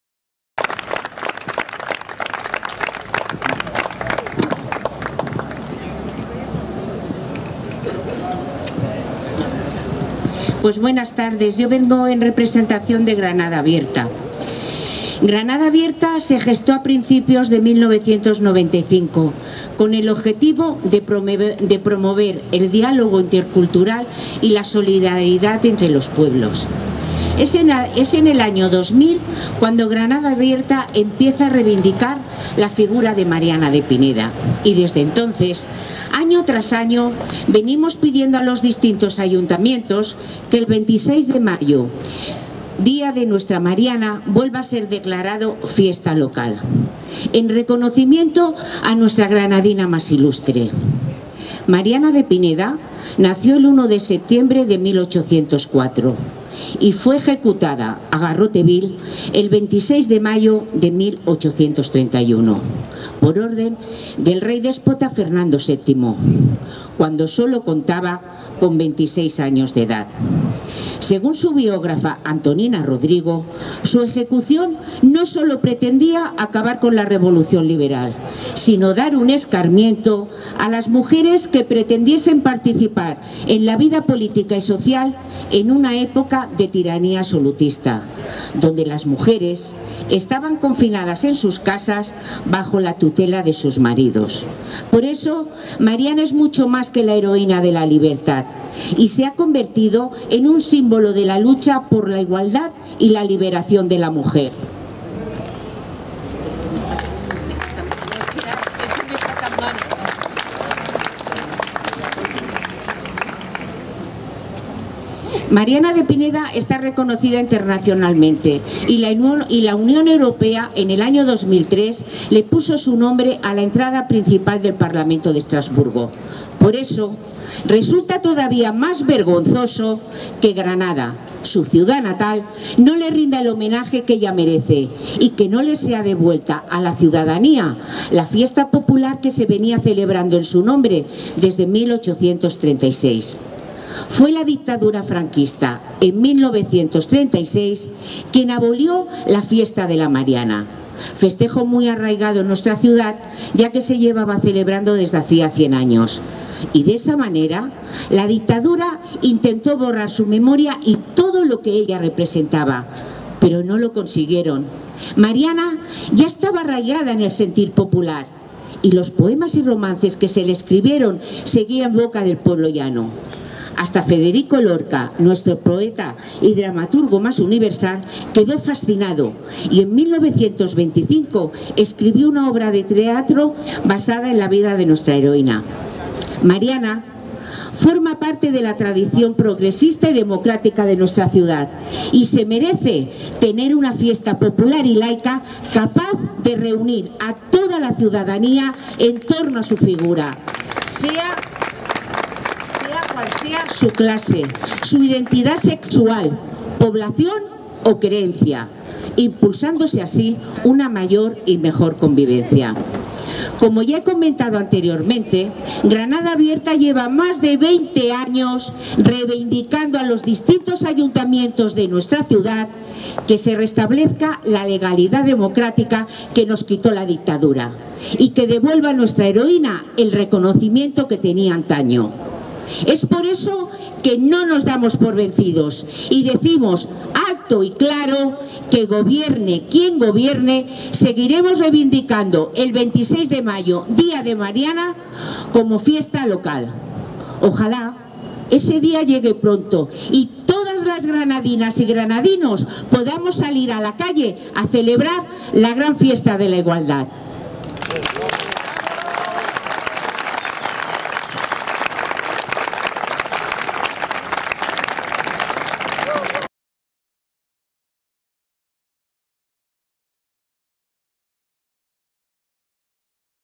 Granada Abierta en la Cumbre Social de Granada
Consuelo Fernández, portavoz de Granada Abierta ha participado en el Encuentro Estatal de la Cumbre Social de Granada. Estas han sido sus palabras